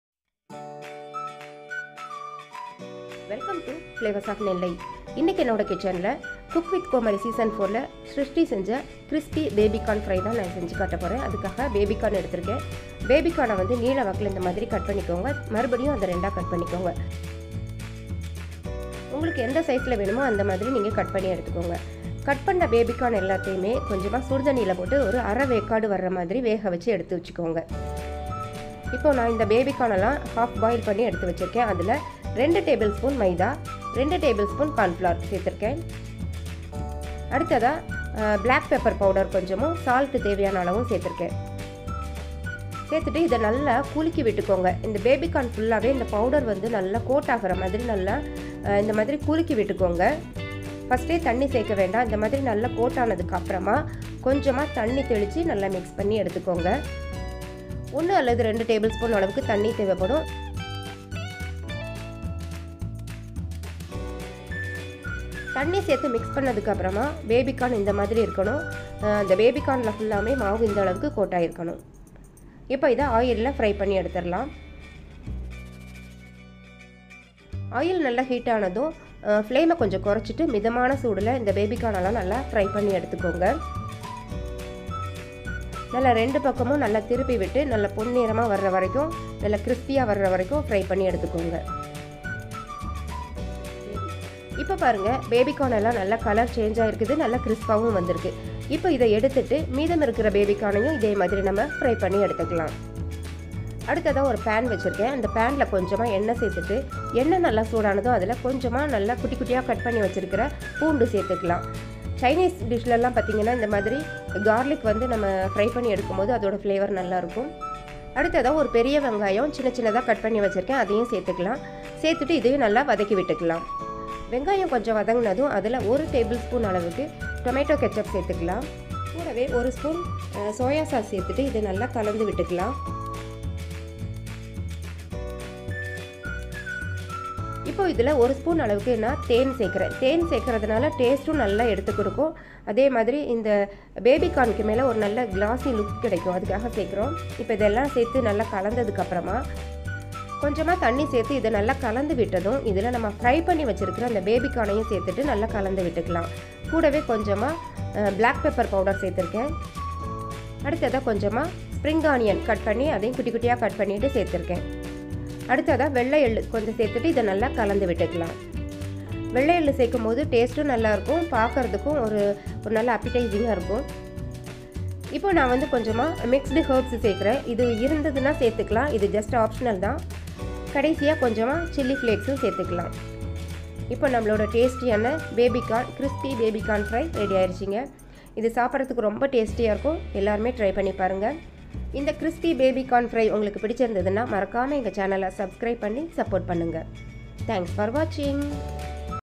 Crispy Baby Corn recipe under 10 mins! | Snack Recipes
Check out the full recipe of this CrispyBabyCorn in chinese style, which can be prepared under 10 mins! Super-easy for beginners and perfect for evening snack and part appetizers, check this recipe done by srushti of cookuwithcomali4 Easy to prepare, delicious and healthy recipe with less ingredients, give a try and share your comments!!

For many more amazing recipes...!